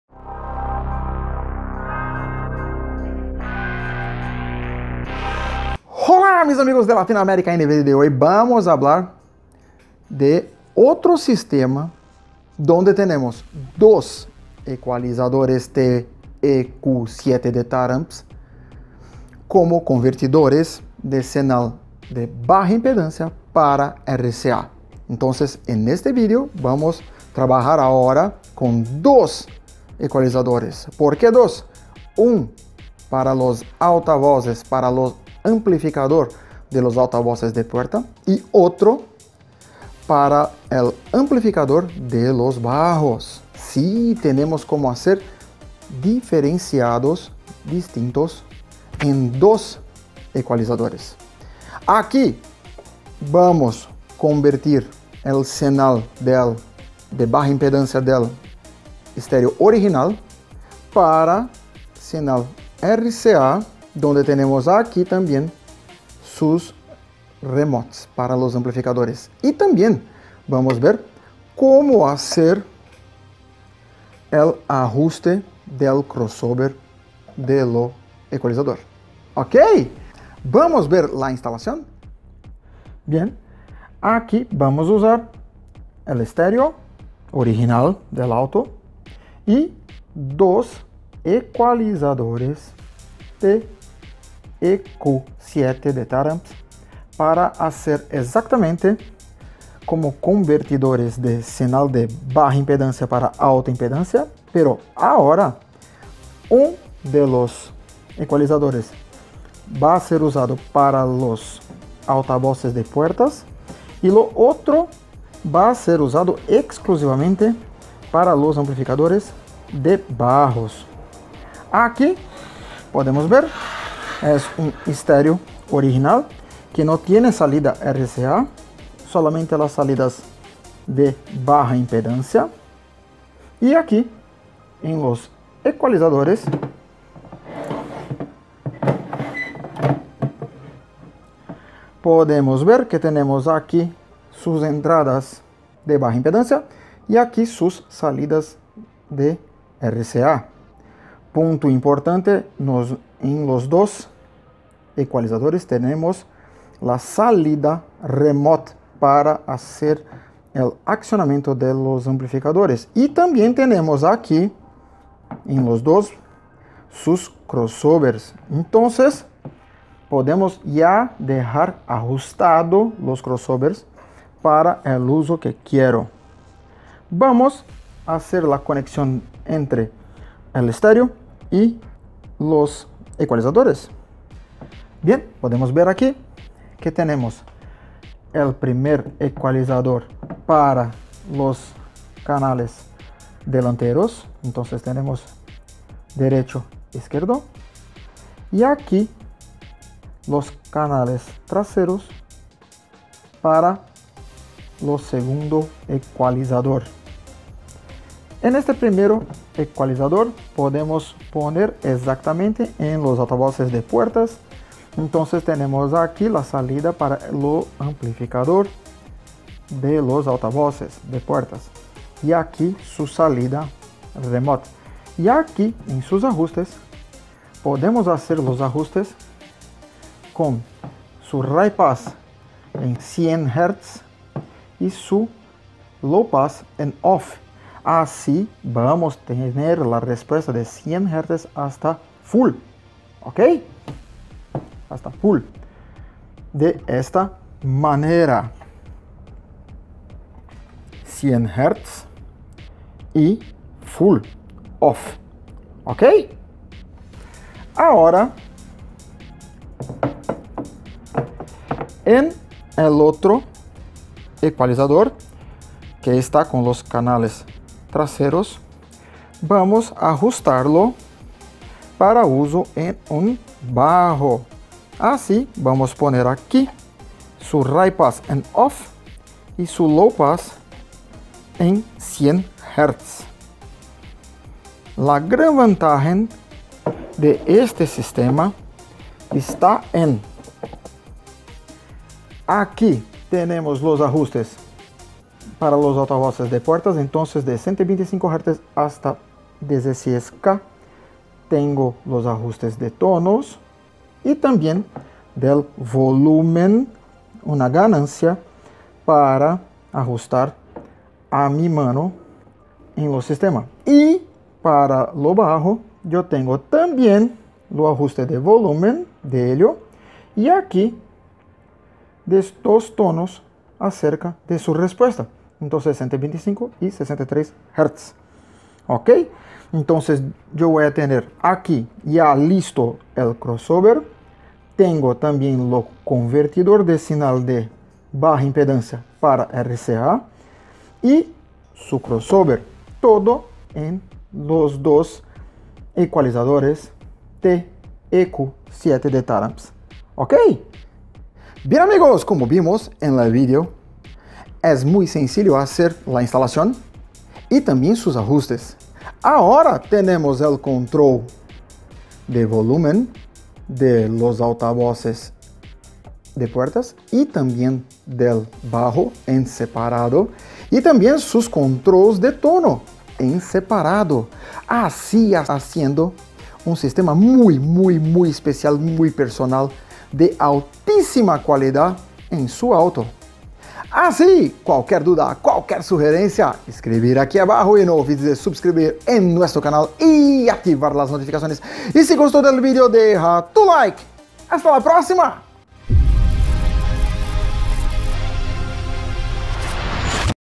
TECH INFO #58 - USO DEL ECUALIZADOR GRAFICO TARAMPS TEQ7 EN RADIO ORIGINAL - PARTE 2
En este video, vamos hablar de la instalación de dos Ecualizadores Graficos de 7 Bandas Taramps TEQ7 en una Radio original del auto, como convertidores de señal de baja impedancia para señal de alta impedancia (RCA) para sistemas con Amplificador para Bajos y Amplificador para los altavoces de puertas.